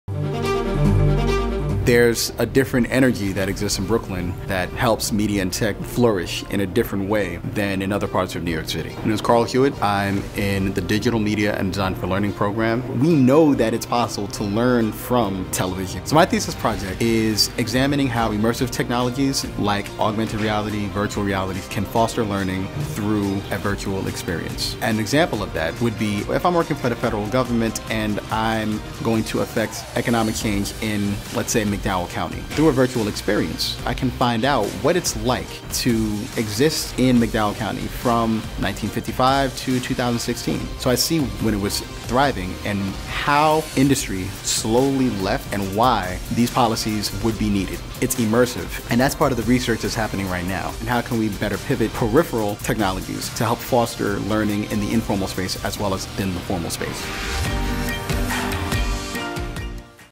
NYU in Brooklyn: Using Virtual Augmented Reality
A passion for educating in informal and formal spaces brought Carl Hewitt to Digital Media Design in Brooklyn—which he feels has a special energy that helps media and tech flourish differently than other parts of New York—to apply immersive digital technologies like AR and VR in fostering learning.